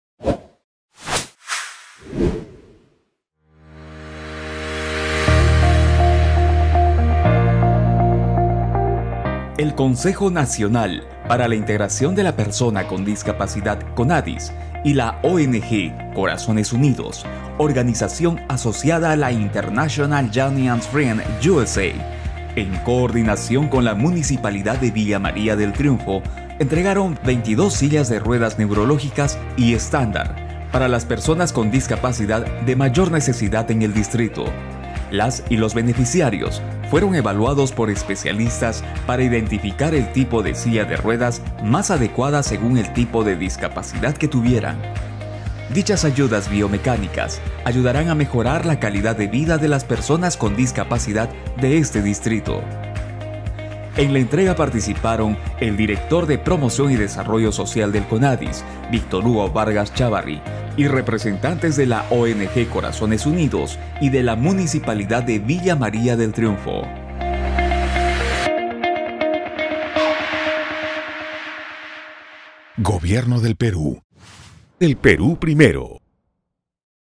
Conadis y Corazones Unidos entregan sillas de ruedas a la Municipalidad de Villa María del Triunfo.
El Consejo Nacional para la Integración de la Persona con Discapacidad (CONADIS) participó, en el acto de entrega de 22 sillas de ruedas a la Oficina Municipal de la Persona con Discapacidad OMAPED de Villa María del Triunfo con la finalidad de atender las necesidades de las personas con discapacidad del distrito.

Durante la entrega de sillas de ruedas que fueron gestionadas por CONADIS ante la ONG Corazones Unidos, participó el Dr. Víctor Hugo Vargas Chávarri, director de la Dirección de Promoción y Desarrollo Social del CONADIS, así como los voluntarios que se encargaron del acondicionamiento de las sillas entregadas y que conforman la organización no gubernamental antes mencionada

Cabe mencionar que las 22 sillas entregadas son de tipo neurológicas y estándar, y cuya selección, recopilación de documentos de los beneficiarios, traslado, espacio adecuado para la entrega, apoyo con terapistas, artículos de higiene, equipos de protección y otros, estarán a cargo de la OMAPED de Villa María del Triunfo.